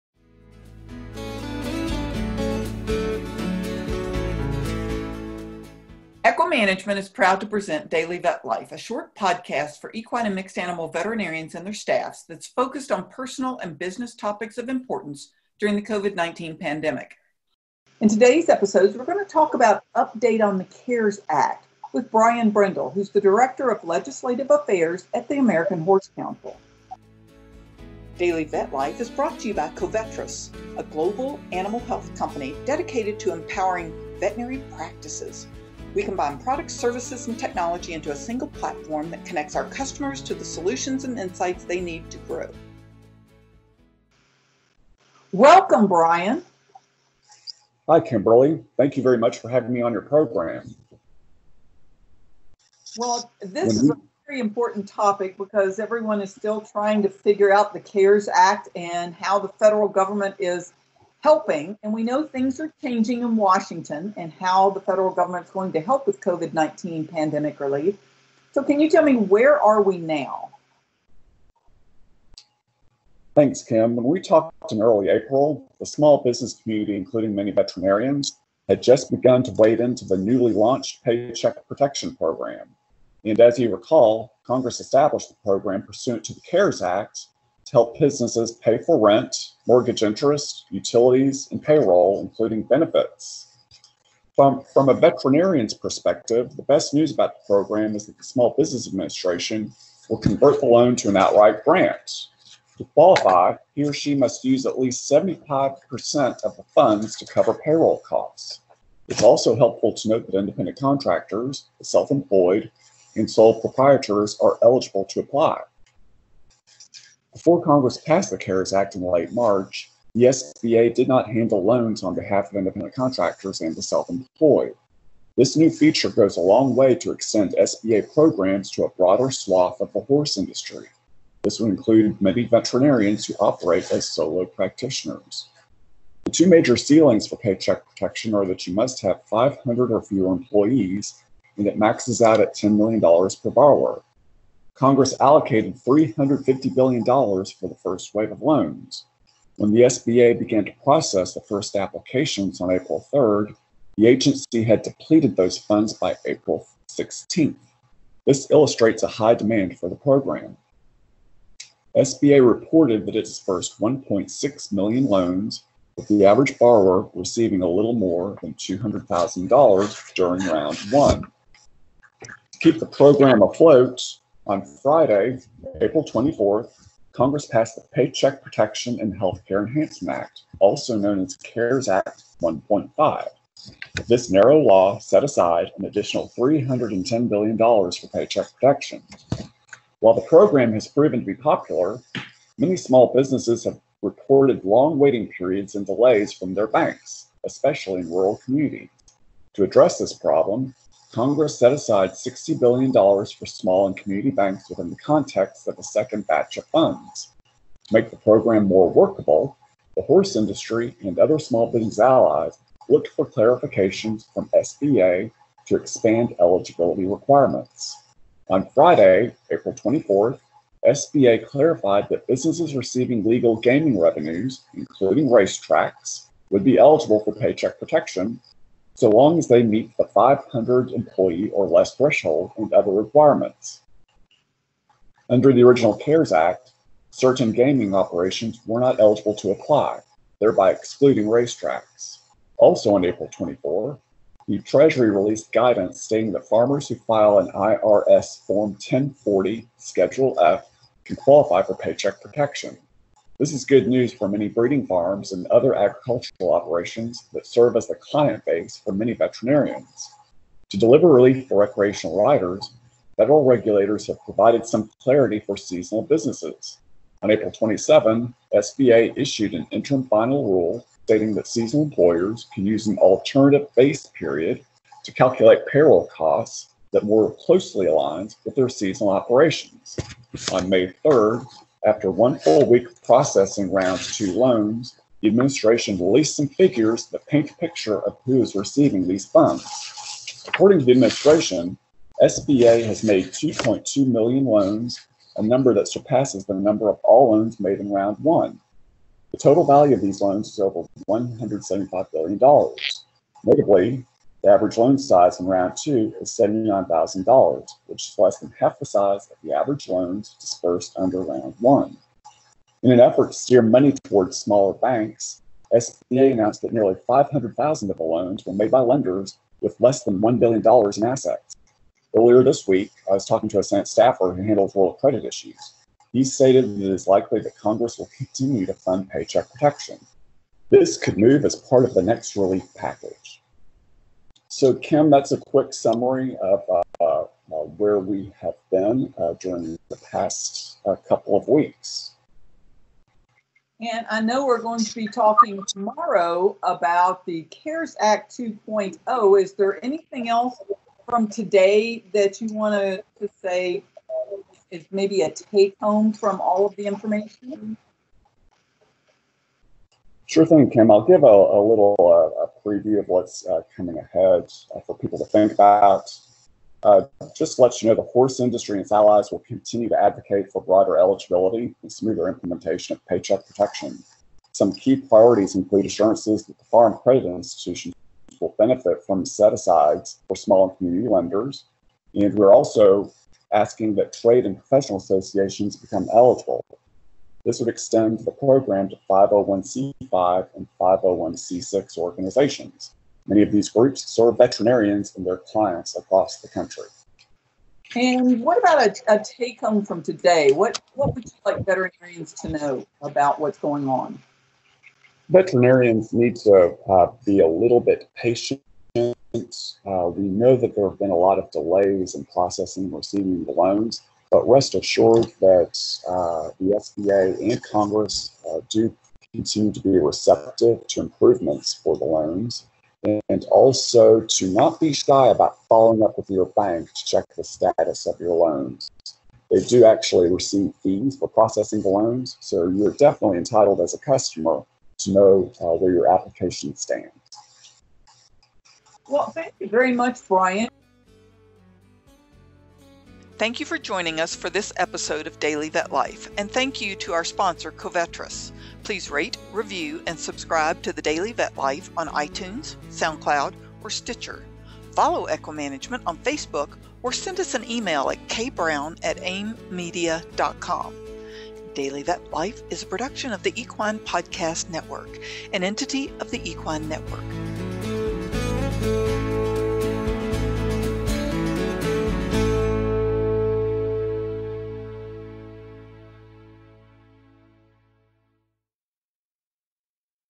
Update on the CARES Act
This episode covers changes to the CARES Act that affect veterinarians.